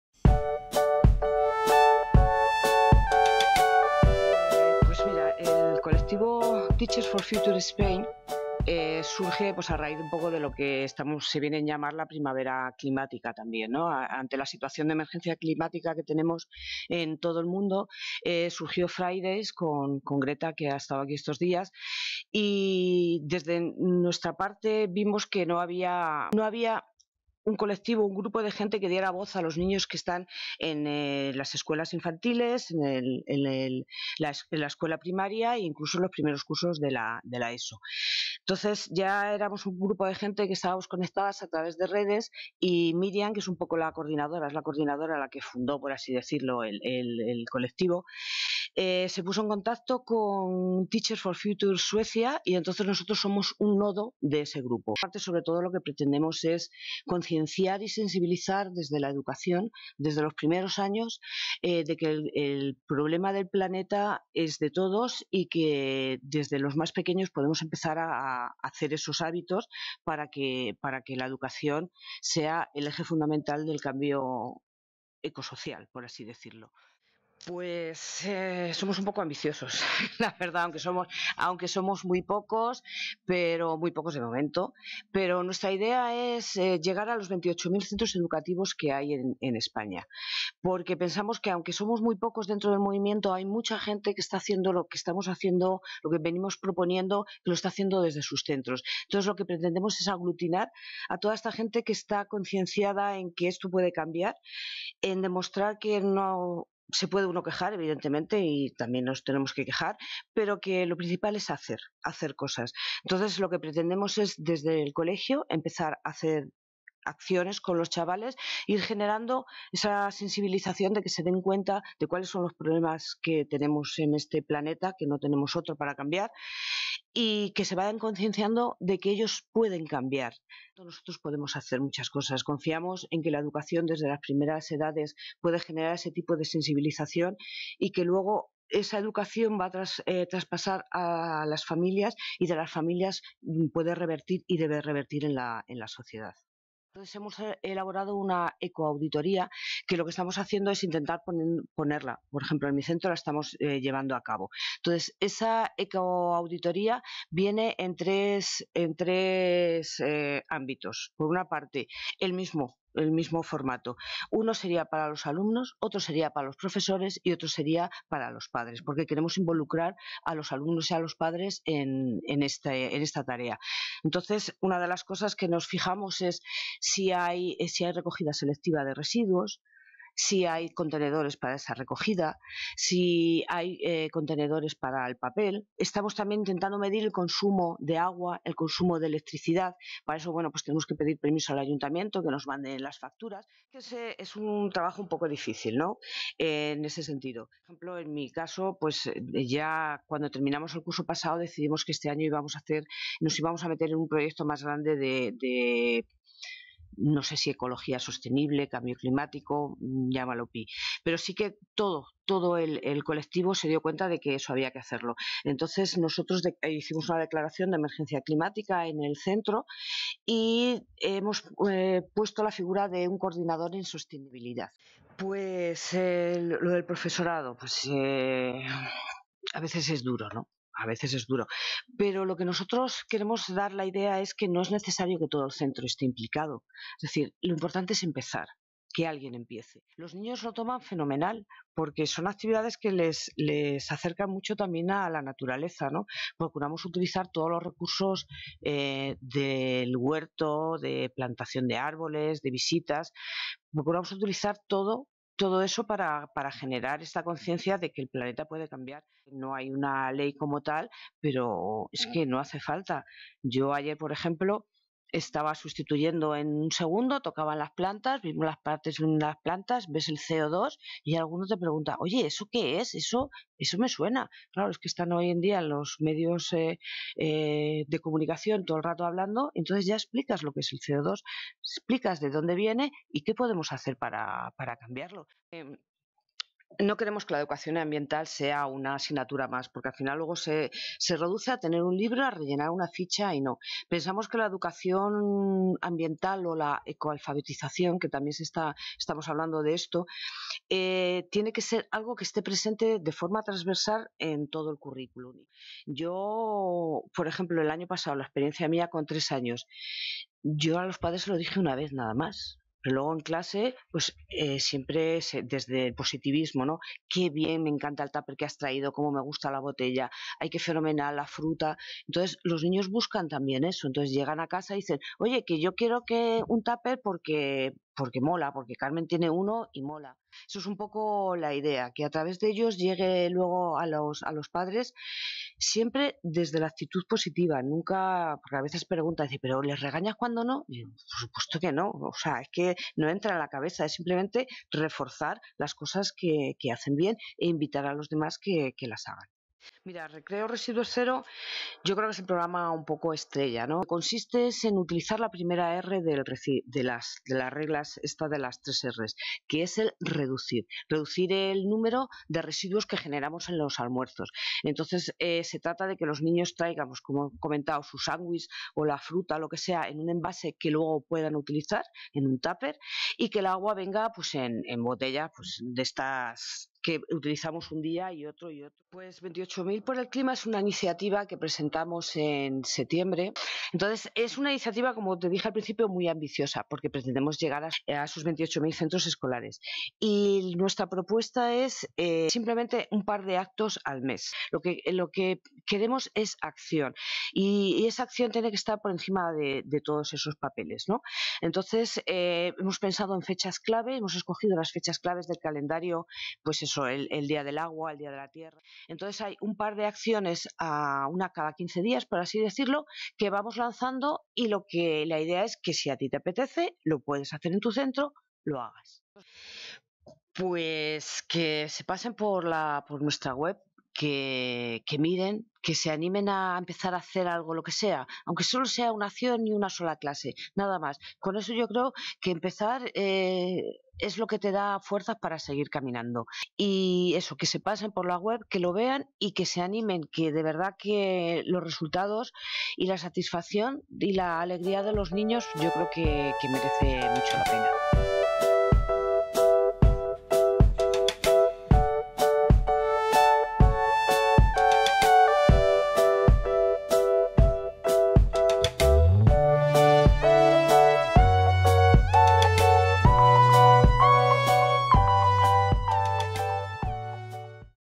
EDUcharla 2_vc: "Cuidando el planeta desde las aulas". Versión corta
Vídeo resumen de 8 minutos de duración sobre la EDUcharla "Cuidando el planeta desde las aulas". Con la participación de la profesora de primaria y miembro de "Teachers for Future" Carmen Gloder Ramos.